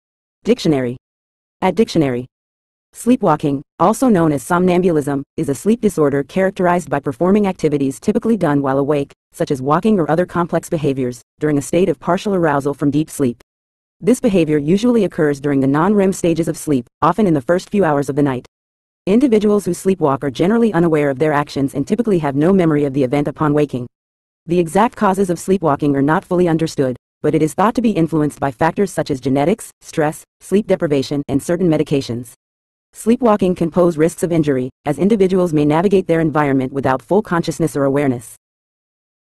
Sleepwalking Meaning In English
Sleepwalking, also known as somnambulism, is a sleep disorder characterized by performing activities typically done while awake, such as walking or other complex behaviors, during a state of partial arousal from deep sleep. This behavior usually occurs during the non-REM stages of sleep, often in the first few hours of the night. Individuals who sleepwalk are generally unaware of their actions and typically have no memory of the event upon waking. The exact causes of sleepwalking are not fully understood, but it is thought to be influenced by factors such as genetics, stress, sleep deprivation, and certain medications. Sleepwalking can pose risks of injury, as individuals may navigate their environment without full consciousness or awareness.